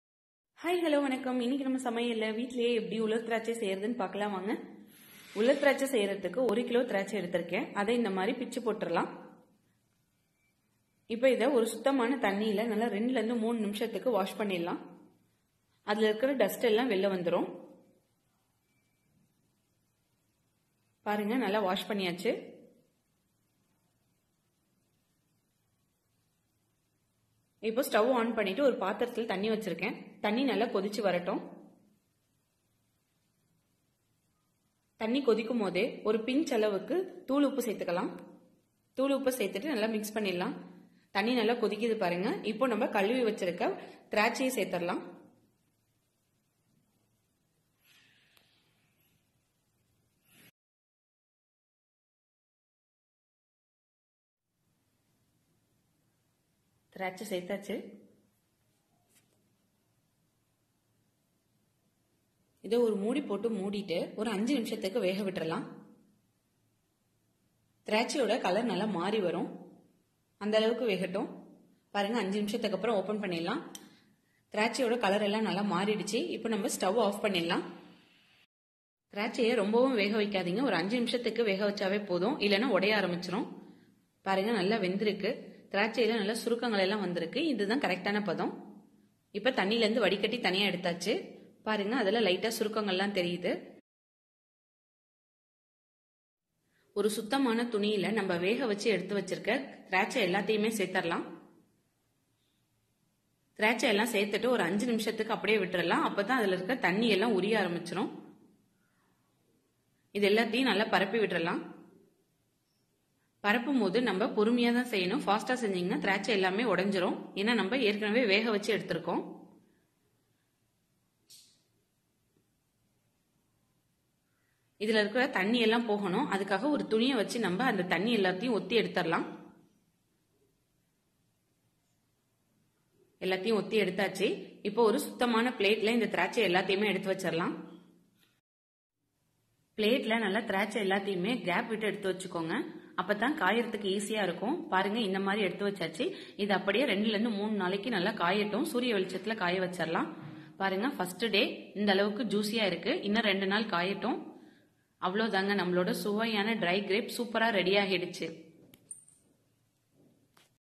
How to make Raisins at home | Dry Grapes Recipe In Tamil |உலர்திராட்சை சுலபமாக செய்யலாம்
In this video we will see how to make tasty and easy home made raisins / Dry Grapes at home in tamil.

Ingredients:

1 kg Green Grapes
water to Boil
pinch of salt
cloth to drain water

This channel is to give you different and tasty Recipe with simple home made ingredients.